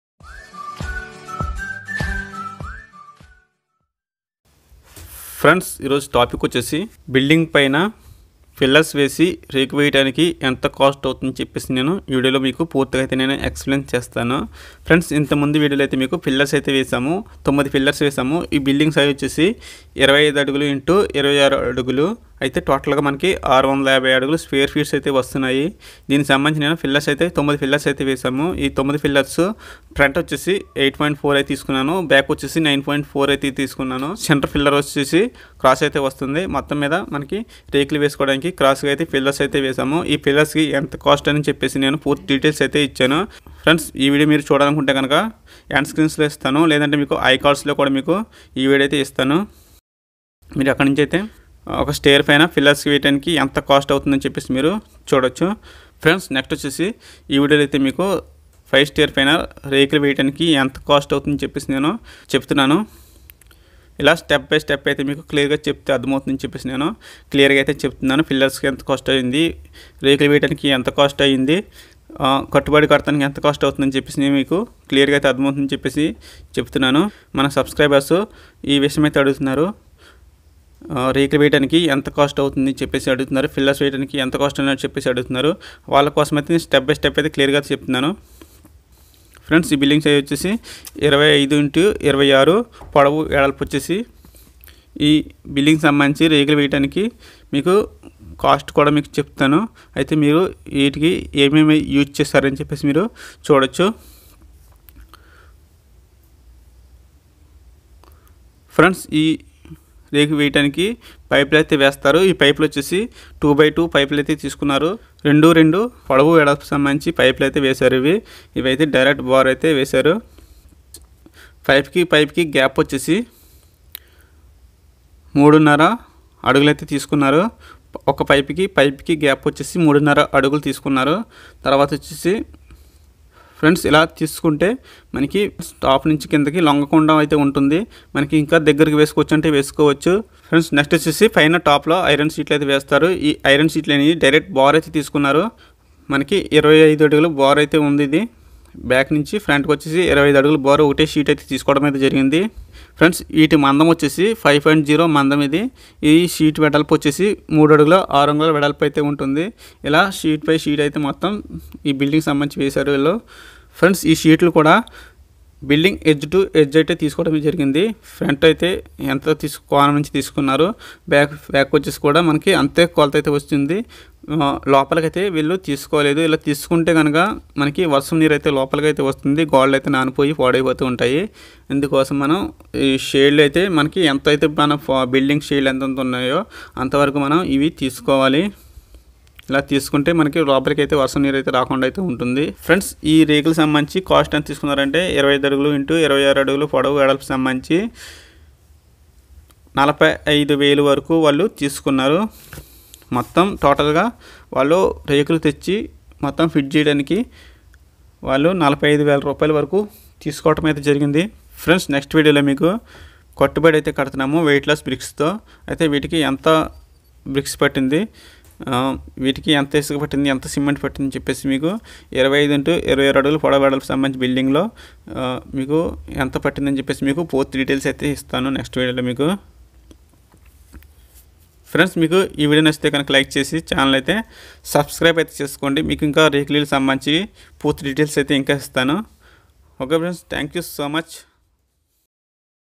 rekula house rekula cost Telugu
Rekula illu rekula cost telugu
రేకుల షెడ్డు పైన కేకులు వేయడానికి ఎంత ఖర్చు అవుతుంది ఫుల్లు డీటెయిల్స్ ఈ వీడియో లో ఉంటాయి